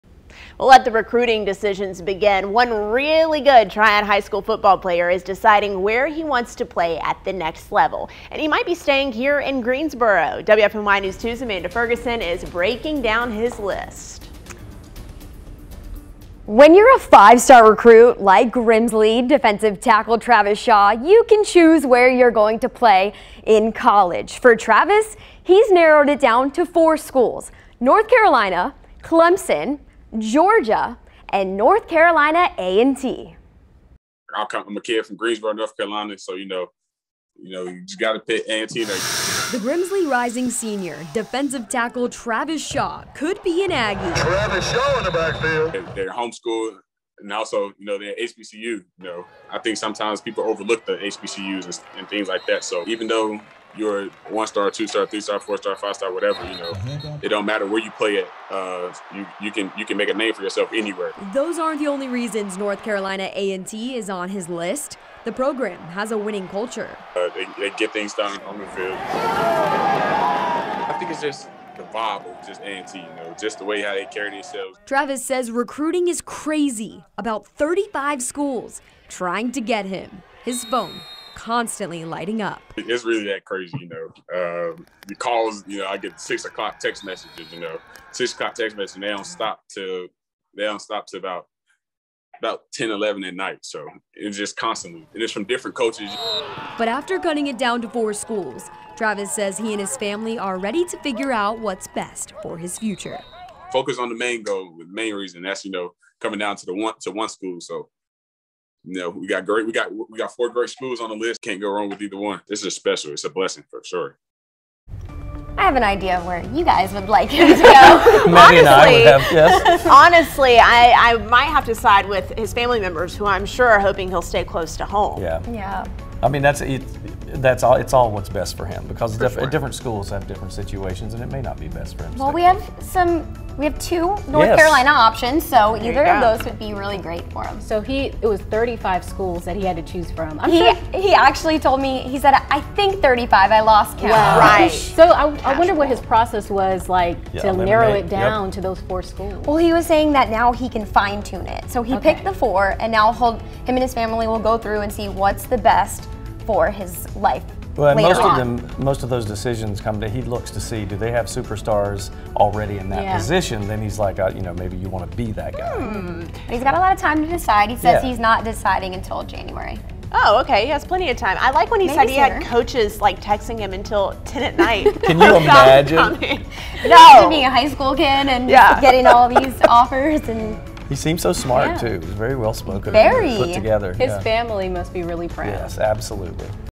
Grimsley High School football player narrows college pick down to four schools
Travis Shaw is a 5-star recruit who has narrowed his college pick down to four schools, including NC A&T. Story